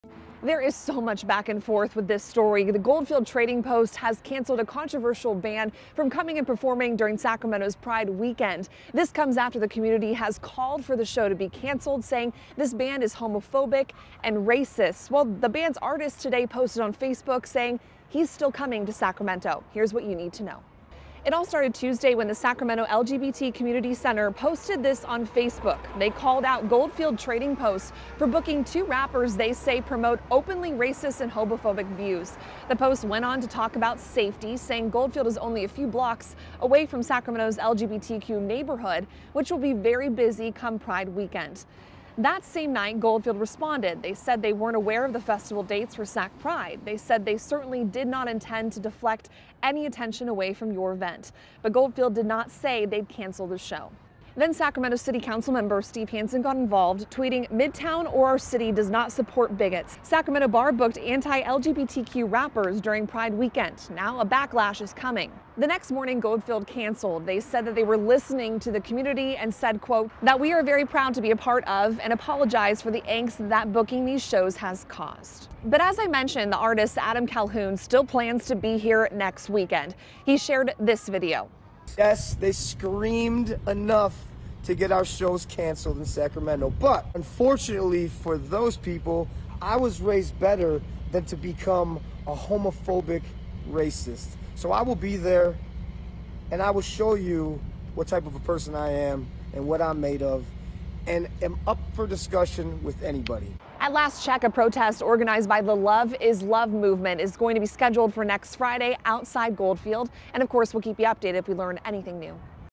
Goldfield Trading Post cancels controversial shows scheduled during Pride weekend after backlash
After days of backlash, Goldfield Trading Post announced they have decided to cancel the controversial shows featuring Adam Calhoun and Demun Jones during Sacramento Pride Weekend.